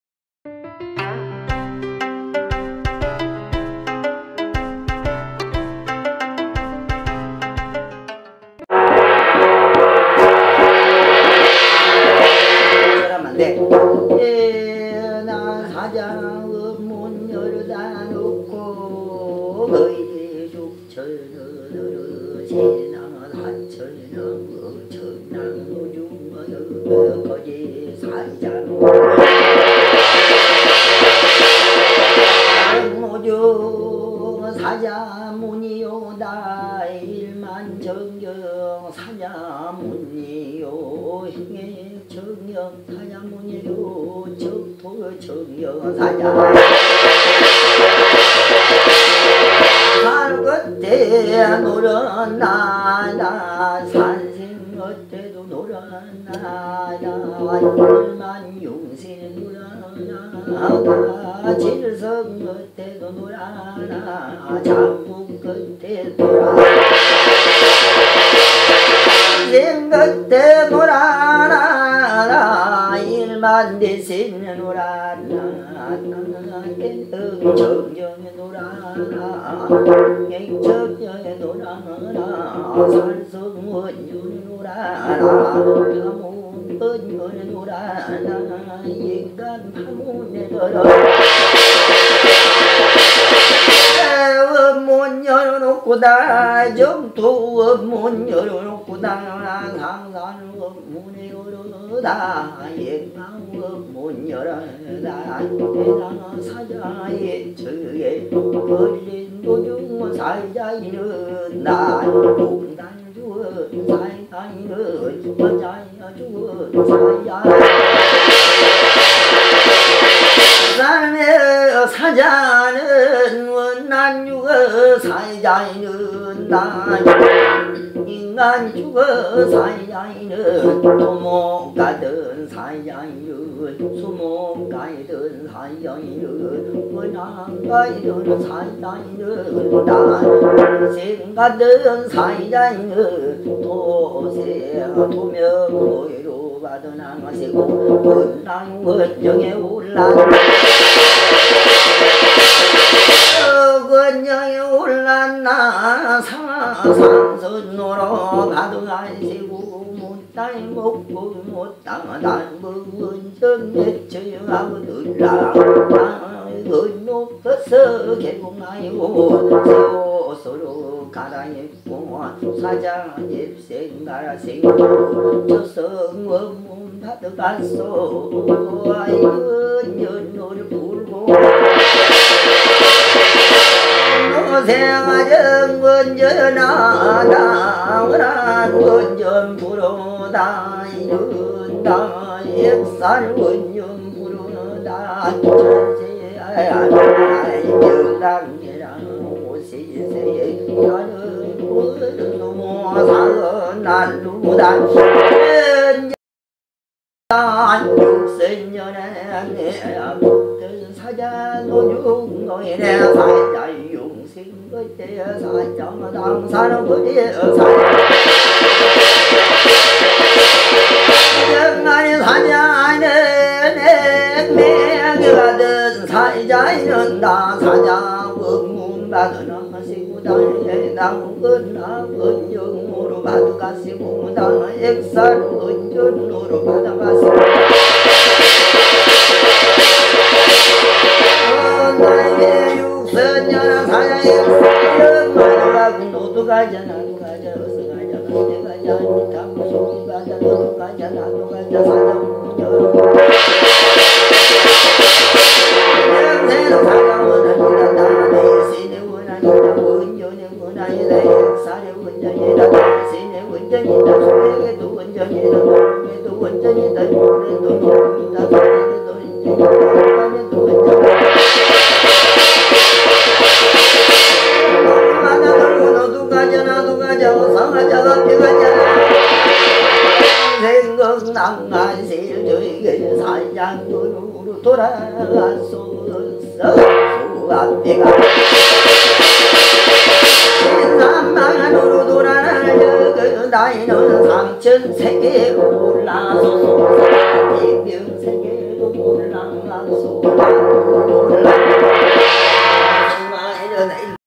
사자 뒷전풀이 사설/경문/염불. [by 부산 강자비 선생님] [I'm mudang/ムダン/백호장군]
굿의 마무리는 뒷전풀이죠. 호칭은 각지역마다 다 다르겠지만,

경상도에서는 뒷전풀이라고 합니다.

 강자비 선생님

부산 금정구 서동

무속인 학원도 운영중.

그외, 문의는
백호장군